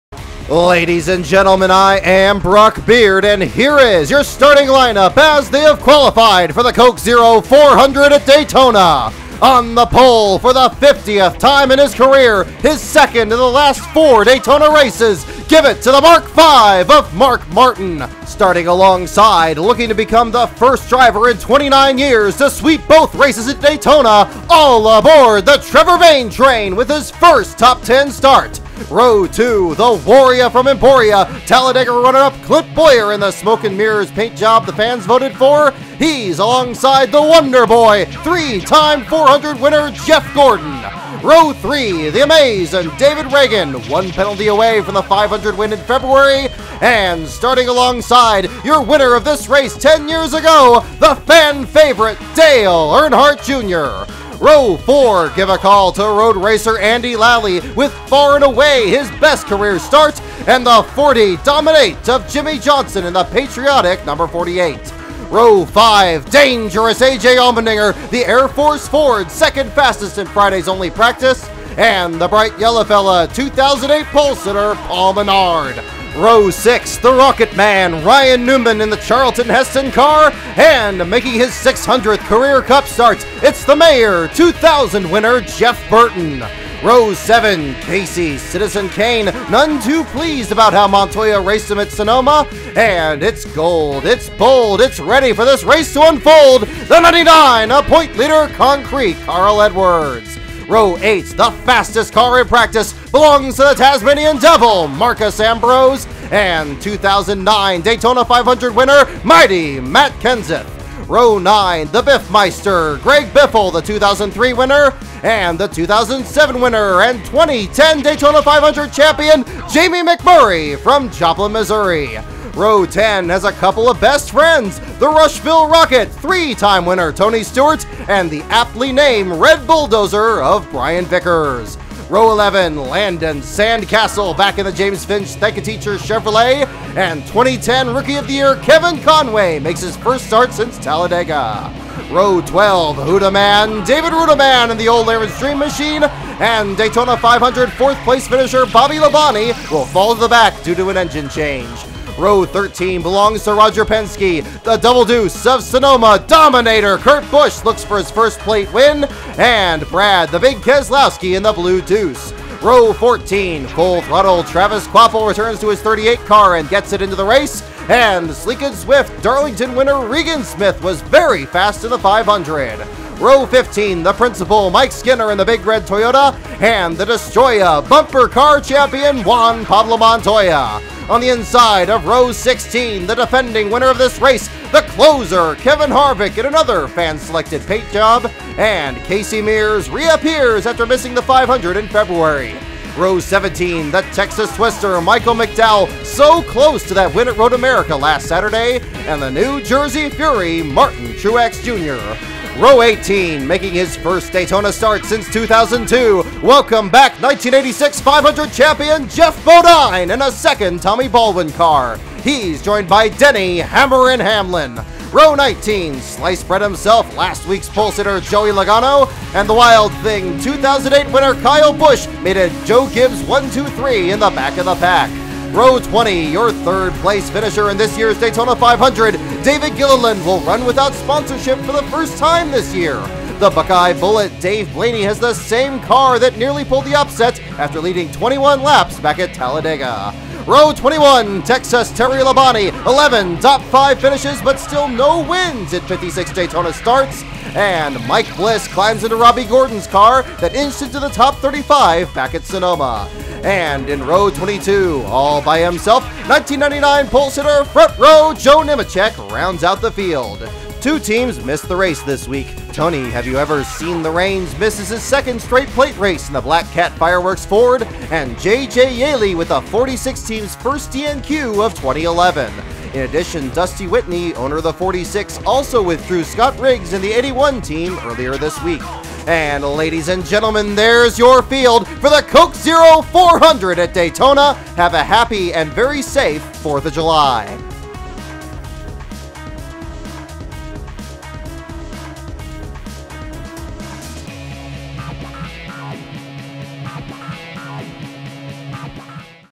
2011 Coke Zero 400 Starting Grid
BAR EXAM-AFFECTED GRID 6 OF 8

TO THE BACK:
#47-Bobby Labonte (engine change)